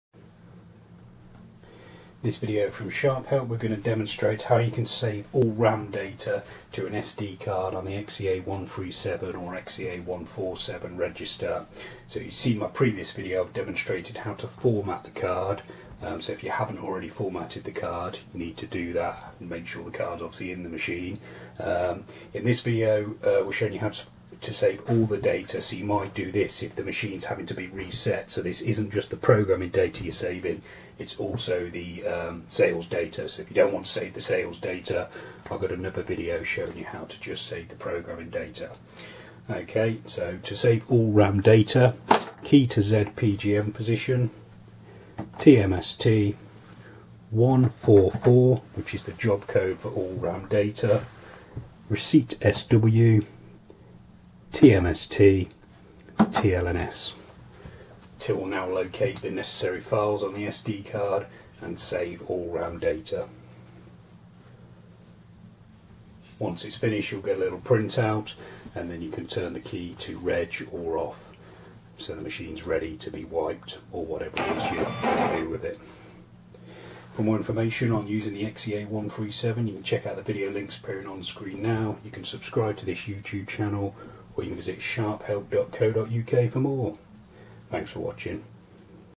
How To Save All RAM Data Sharp XE-A137 / XEA137 / XE-A1478 / XEA147
How to save all RAM data (programming & sales data) to SD card on the Sharp XE-A137 or XE-A147 cash registers.